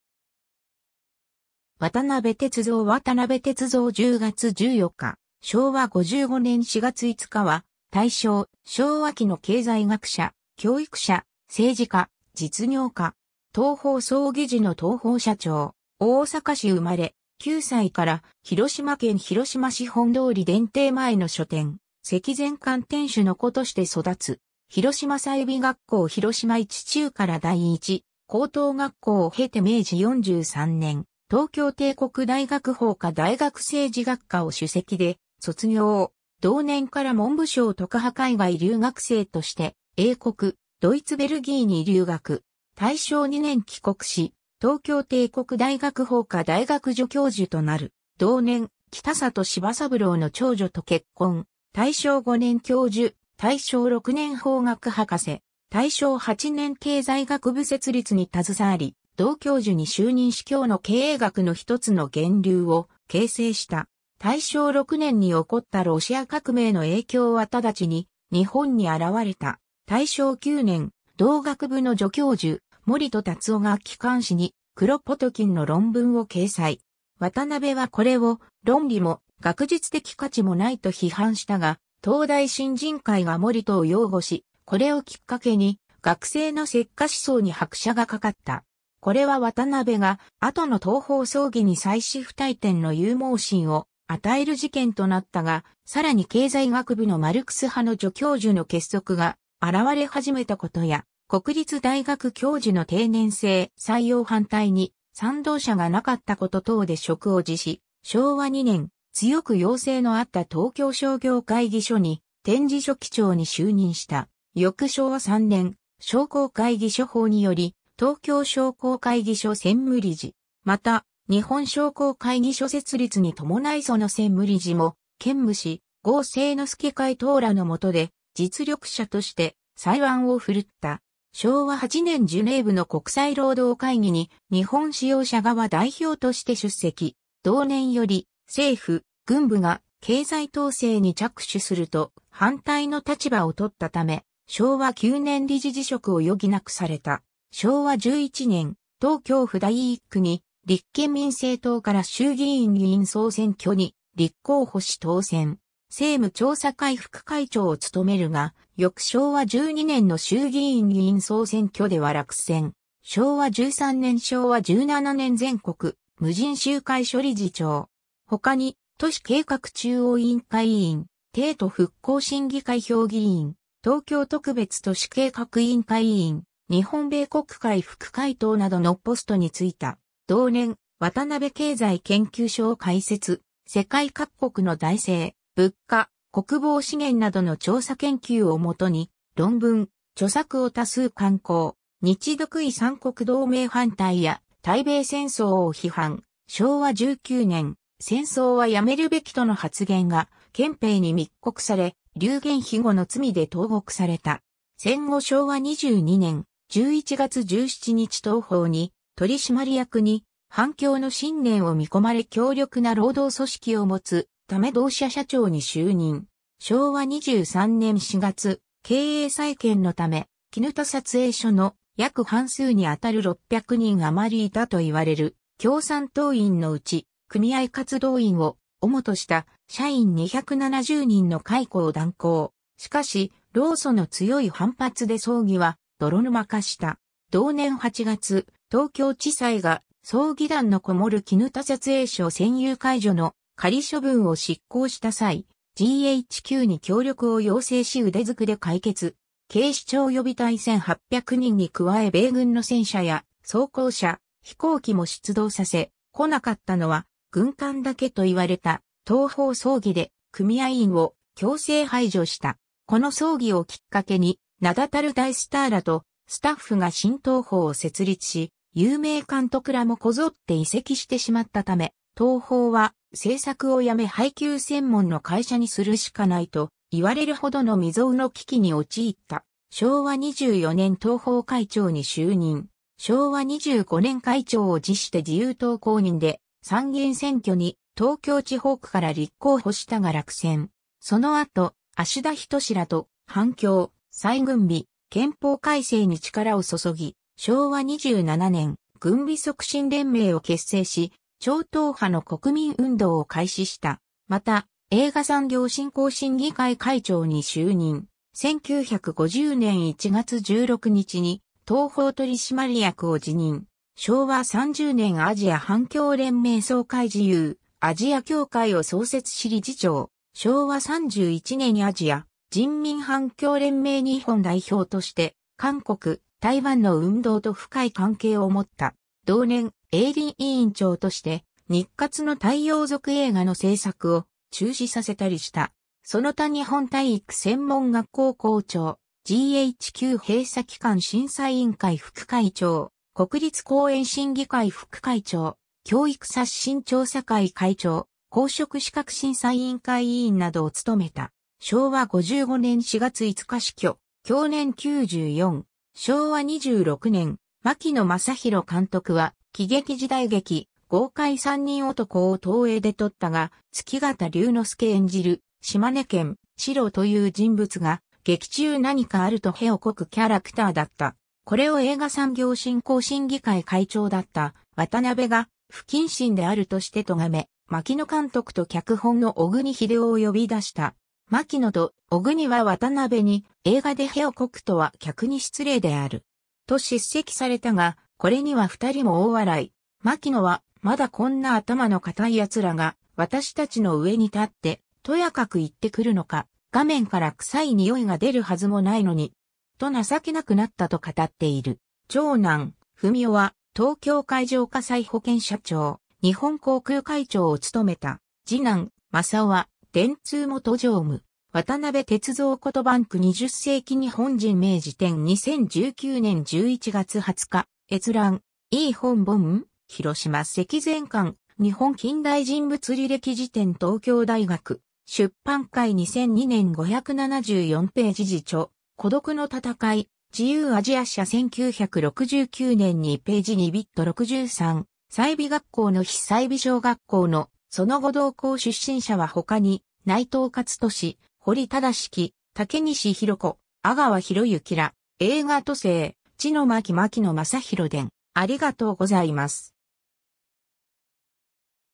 渡辺銕蔵
東京府選出の衆議院議員_(帝国議会)
渡辺銕蔵 渡辺 銕蔵（わたなべ てつぞう、明治18年（1885年）10月14日 - 昭和55年（1980年）4月5日）は、大正・昭和期の経済学者、教育者、政治家、実業家。
東宝争議時の東宝社長。
 大阪市生まれ。
9歳から広島県広島市本通電停前の書店・積善館店主の子として育つ。
 広島済美学校～広島一中～第一高等学校を経て明治43年（1910年）、東京帝国大学法科大学政治学科を首席で卒業。
同年から文部省特派海外留学生として英国・ドイツ・ベルギーに留学。
 大正2年（1913年）帰国し東京帝国大学法科大学助教授となる。
同年北里柴三郎の長女と結婚。
大正5年（1916年）教授、大正6年（1917年）法学博士。
大正8年（1919年）経済学部設立に携わり同教授に就任し今日の経営学の一つの源流を形成した。
大正6年に起こったロシア革命の影響は直ちに日本に現れた。
大正9年（1920年）、同学部の助教授・森戸辰男が機関誌にクロポトキンの論文を掲載（森戸事件）。
渡辺はこれを論理も学術的価値もないと批判したが、東大新人会が森戸を擁護し、これをきっかけに学生の赤化思想に拍車がかかった。
これは渡辺が後の東宝争議に際し不退転の勇猛心を与える事件となったが、さらに経済学部のマルクス派の助教授の結束が現われ始めたことや、国立大学教授の停年制採用反対に賛同者が無かったこと等で職を辞し、昭和2年（1927年）、強く要請のあった東京商業会議所に転じ書記長に就任した。
翌昭和3年（1928年）、商工会議所法により東京商工会議所専務理事。
また日本商工会議所設立に伴いその専務理事も兼務し、郷誠之助会頭らの下で実力者として才腕を振るった。
昭和8年（1933年）ジュネーヴの国際労働会議に日本使用者側代表として出席。
同年より政府・軍部が経済統制に着手すると反対の立場をとったため、昭和9年（1934年）理事辞職を余儀なくされた。
 昭和11年（1936年）、東京府第1区に立憲民政党から衆議院議員総選挙に立候補し当選。
政務調査会副会長を務めるが、翌昭和12年（1937年）の衆議院議員総選挙では落選。
 昭和13年（1938年）～昭和17年（1942年）全国無尽集会所（全国相互銀行協会）理事長。
他に都市計画中央委員会委員・帝都復興審議会評議員・東京特別都市計画委員会委員、日本米穀会副会頭などのポストに就いた。
 同年、「渡辺経済研究所」を開設。
世界各国の財政・物価・国防資源などの調査研究を元に論文・著作を多数刊行。
日独伊三国同盟反対や対米戦争を批判、昭和19年（1944年）、「戦争はやめるべき」との発言が憲兵に密告され、流言蜚語の罪で投獄された。
 戦後昭和22年（1947年）、11月17日東宝に取締役に反共の信念を見込まれ強力な労働組織を持つため同社社長に就任。
昭和23年（1948年）4月、経営再建のため、砧撮影所の約半数にあたる600人余りいたといわれる共産党員のうち、組合活動員を主とした社員270人の解雇を断行。
しかし労組の強い反発で争議は泥沼化した。
同年8月、東京地裁が争議団のこもる砧撮影所占有解除の仮処分を執行した際、GHQに協力を要請し腕ずくで解決。
警視庁予備隊1800人に加え米軍の戦車や装甲車、飛行機も出動させ「こなかったのは軍艦だけ」といわれた東宝争議で、組合員を強制排除した。
この争議をきっかけに名だたる大スターらとスタッフが新東宝を設立し、有名監督らもこぞって移籍してしまったため、東宝は制作をやめ配給専門の会社にするしかないといわれるほどの未曾有の危機に陥った。
昭和24年（1949年）東宝会長に就任。
 昭和25年（1950年）会長を辞して自由党公認で参議院選挙に東京地方区から立候補したが落選。
その後芦田均らと反共・再軍備・憲法改正に力を注ぎ昭和27年（1952年）「軍備促進連盟」を結成し超党派の国民運動を開始した。
また映画産業振興審議会会長に就任。
1950年1月16日に東宝取締役を辞任。
 昭和30年（1955年）アジア反共連盟総会自由アジア協会を創設し理事長。
昭和31年（1956年）アジア人民反共連盟（APACL）日本代表として韓国・台湾の運動と深い関係を持った。
同年、映倫委員長として日活の"太陽族映画"の製作を中止させたりした。
その他日本体育専門学...